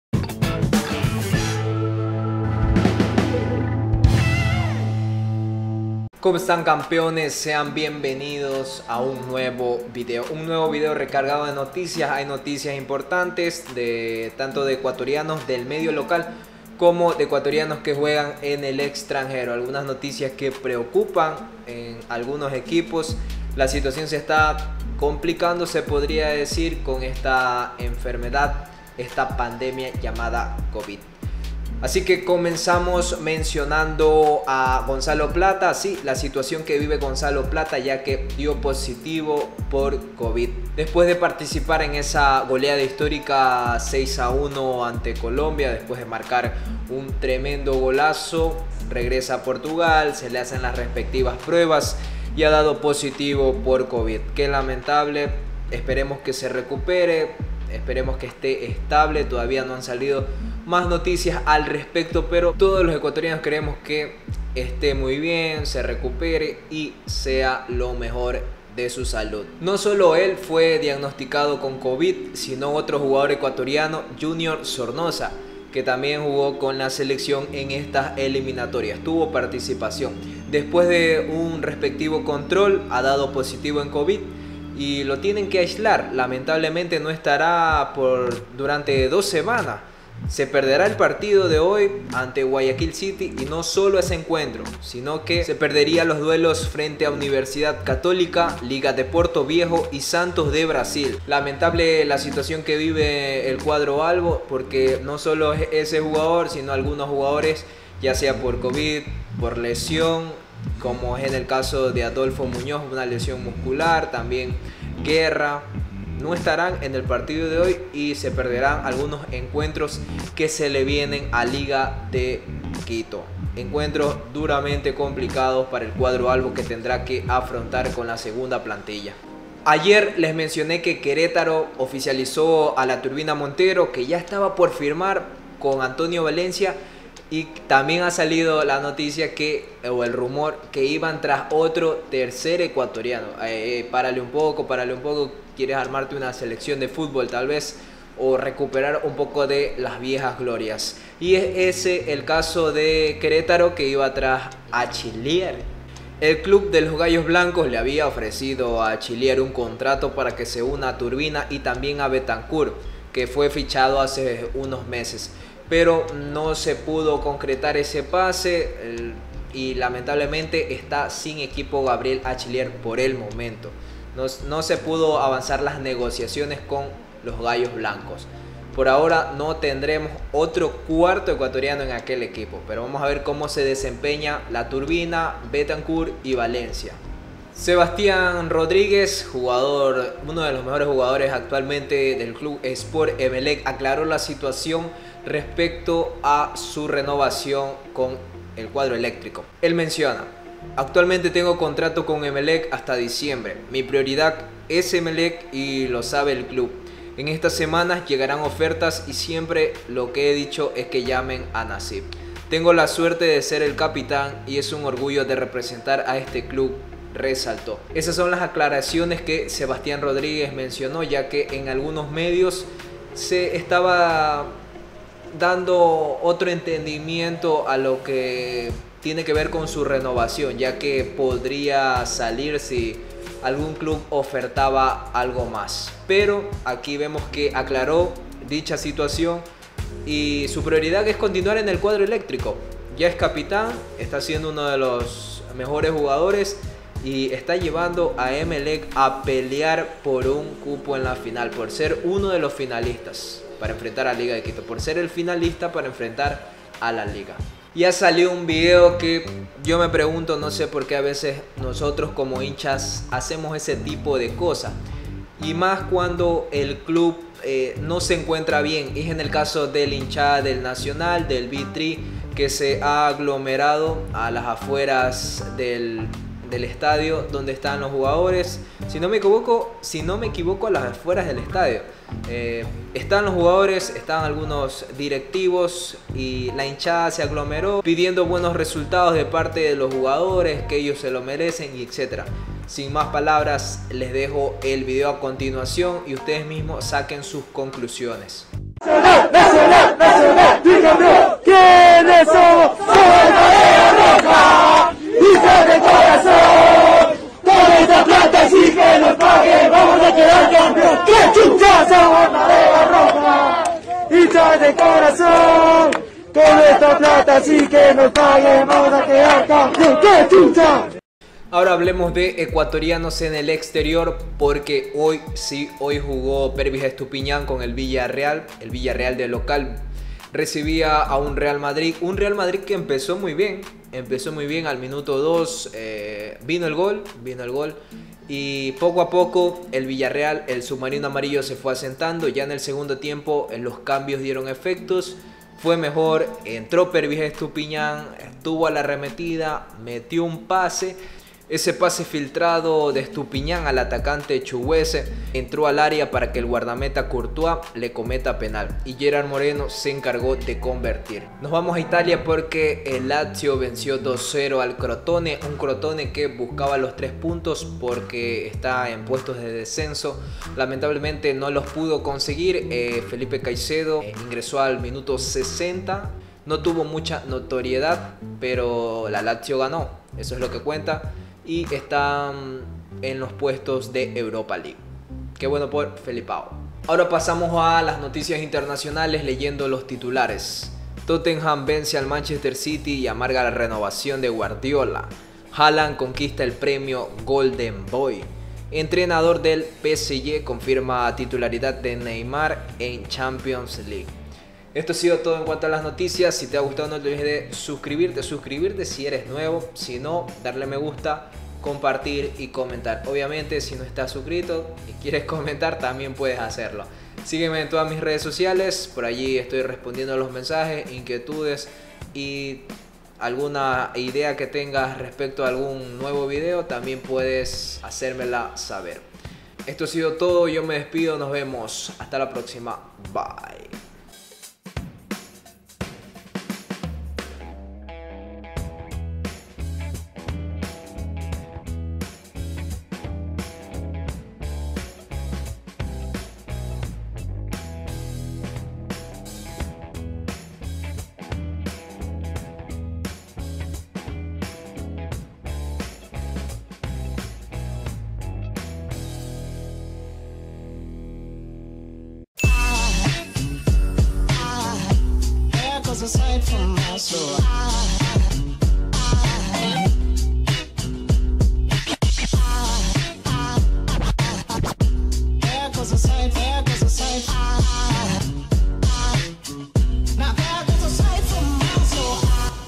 PERVIS LETAL ante REAL MADRID| SEBAS RODRIGUEZ Continuará en EMELEC| MUCHAS bajas en LIGA de Quito!
SÍGUEME